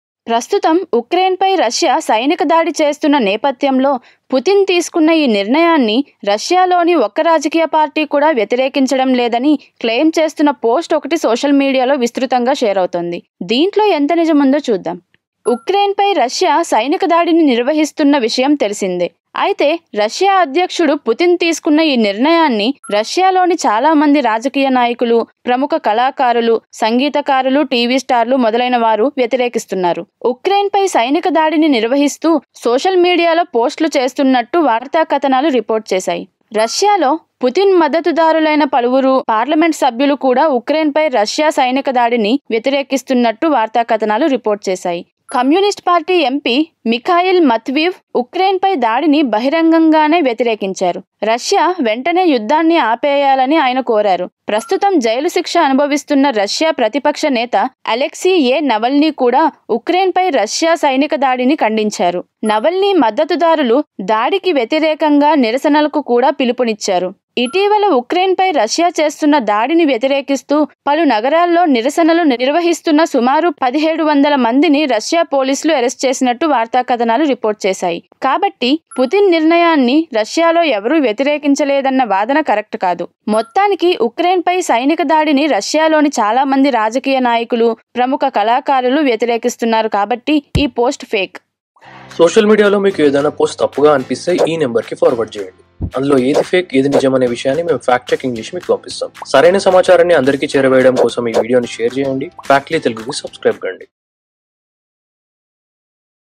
FACT CHECK: ఉక్రెయిన్‌పై సైనిక దాడిని రష్యాలో ఎవరూ వ్యతిరేకించడం లేదా? | Factly Telugu
ప్రస్తుతం ఉక్రెయిన్‌పై రష్యా సైనిక దాడి చేస్తున్న నేపథ్యంలో పుతిన్ తీసుకున్న ఈ నిర్ణయాన్ని రష్యాలోని ఒక్క రాజకీయ పార్టీ కూడా వ్యతిరేకించలేదని షేర్ చేస్తున్న పోస్టులో నిజమెంతో ఈ వీడియో ద్వారా తెలుసుకోండి.